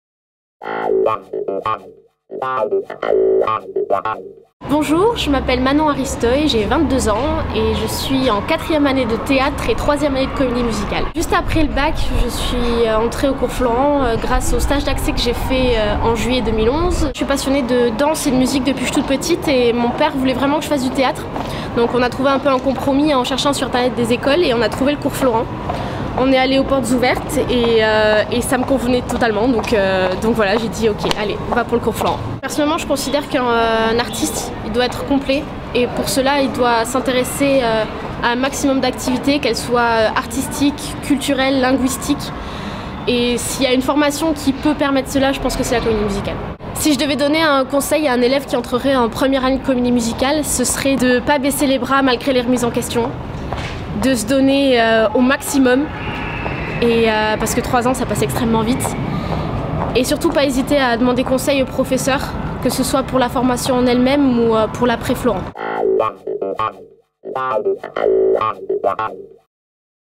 Portrait de Manon Haristoy, élève au Cours Florent Comédie Musicale à Paris
découvrez le portrait de Manon Haristoy, élève au Cours Florent Comédie Musicale à Paris.